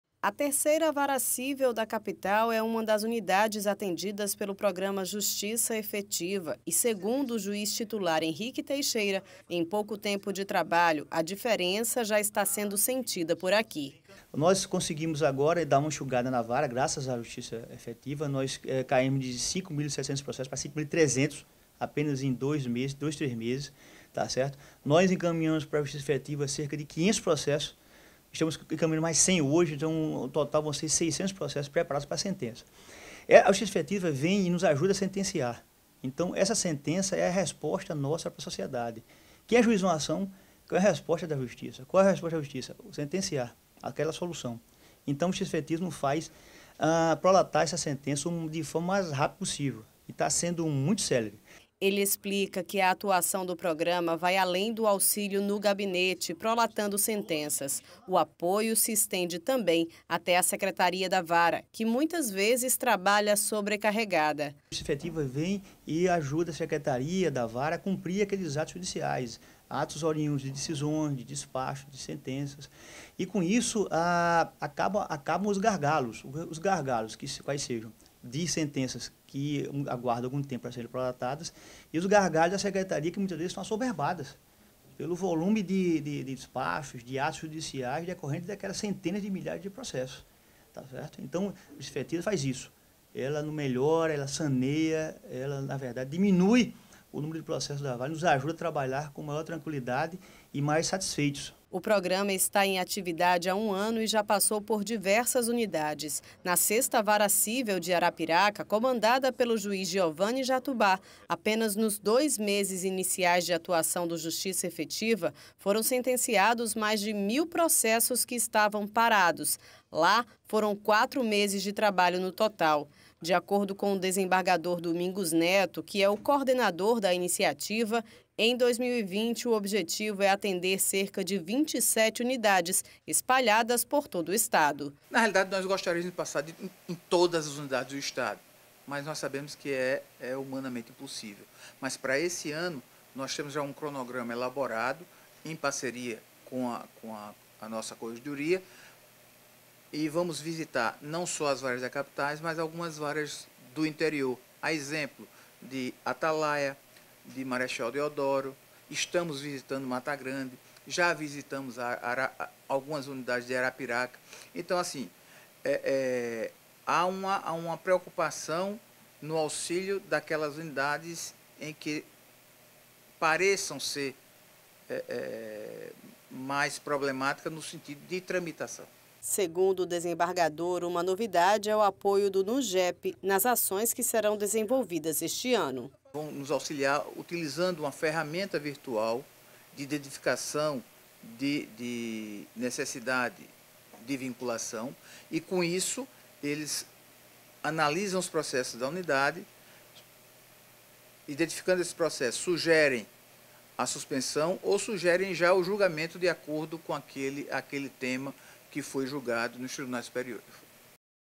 Justiça Efetiva agiliza trabalhos na 3ª Vara Cível da Capital
Em três meses de atuação, o programa diminuiu o número de processos que tramitam na unidade; a ação também terá apoio do Núcleo de Gerenciamento de Precedentes